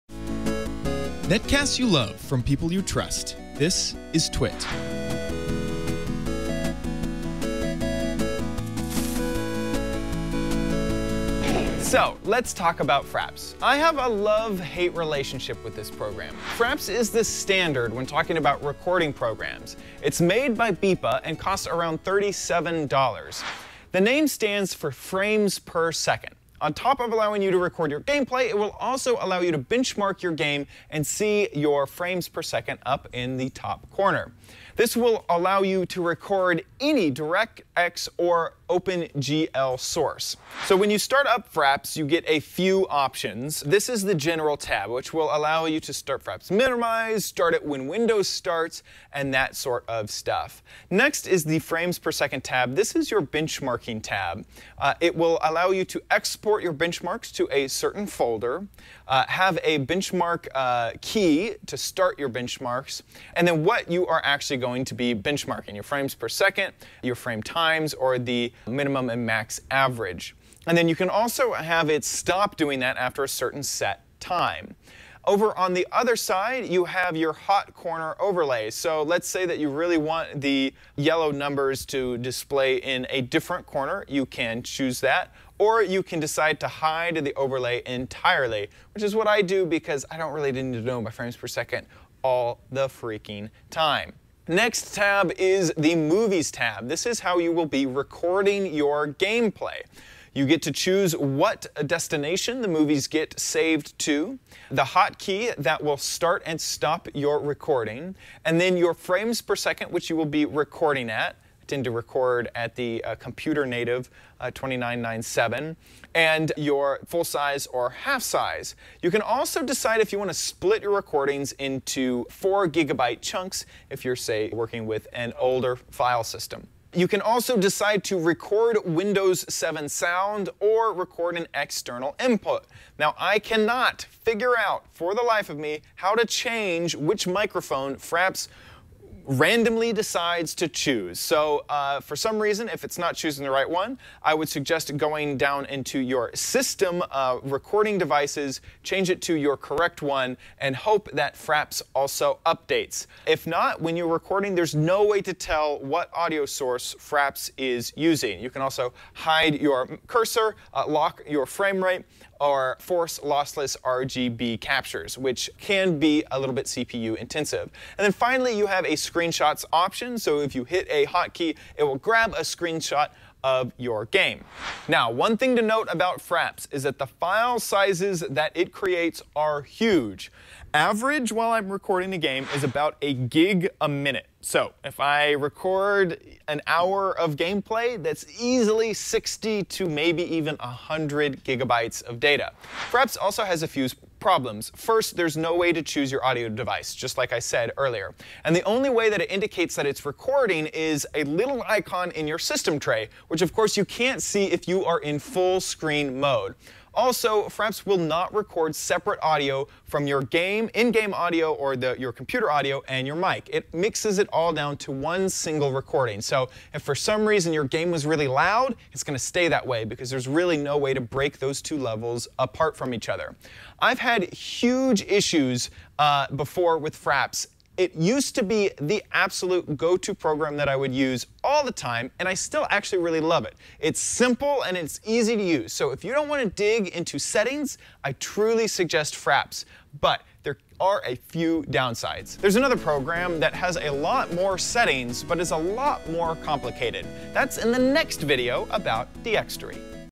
Recording your own videos, FRAPS
Host: Chad "OMGchad" Johnson

We take an overview of Fraps, the simple to use program to record your gameplay.